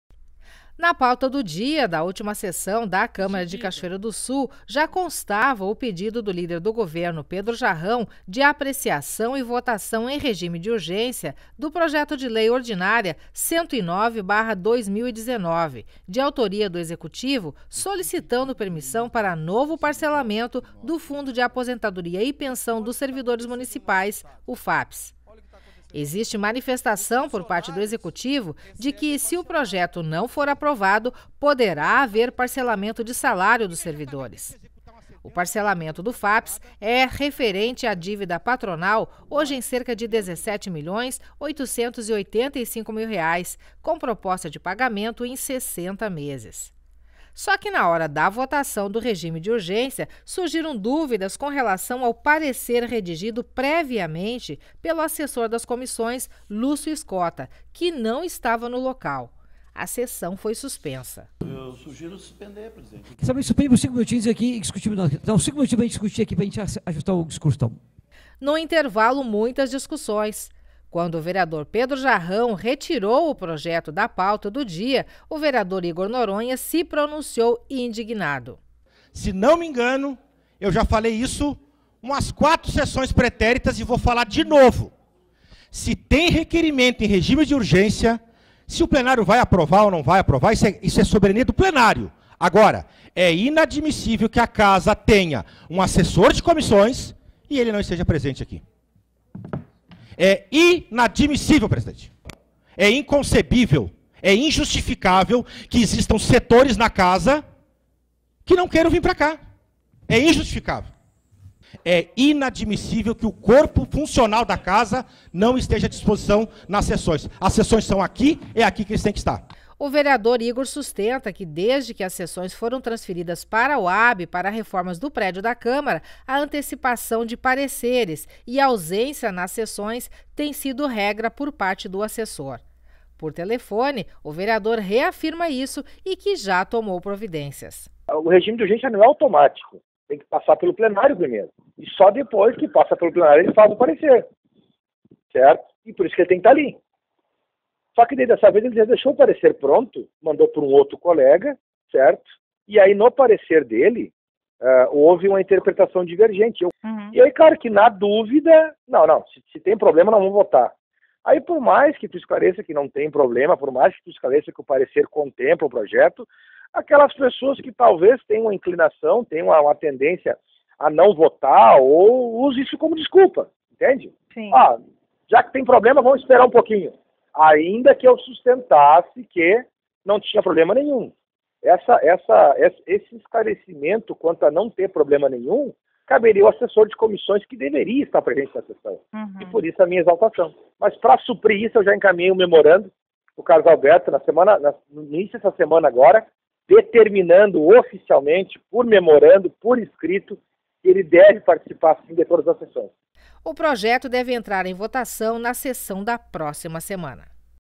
Projeto de parcelamento do fundo de aposentadoria ainda preocupa
Mesmo tendo sido retirado de pauta, servidores se mantêm na expectativa quando ao tema.